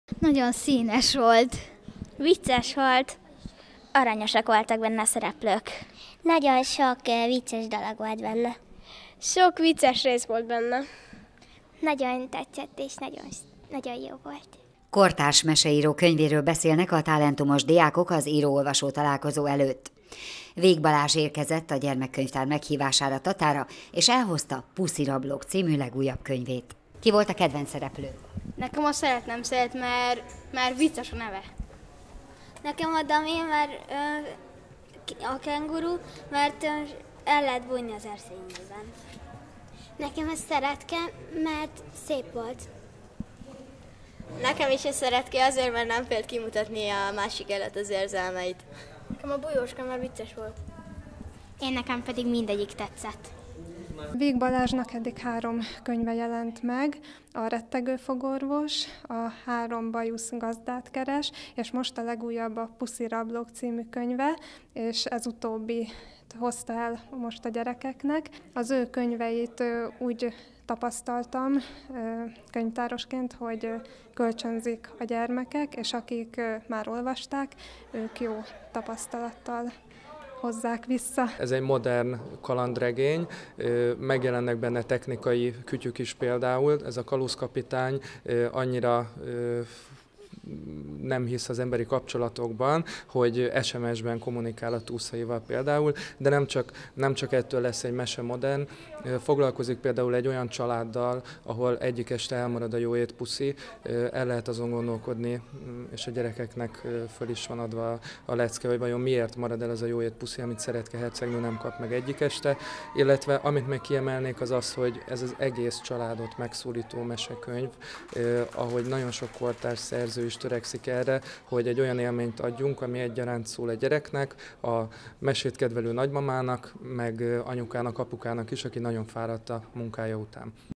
141105 Híradó 03 Vig Balázzsal találkozhattak ma az érdeklődők a gyermekkönyvtárban
Vig Balázs íróval találkozhattak ma az érdeklődők a Móricz Zsigmond Városi Könyvtár Gyermekkönyvtárában. A népszerű kortárs meseíró „Puszirablók” című könyvéről beszélgettek író-olvasó találkozó keretében.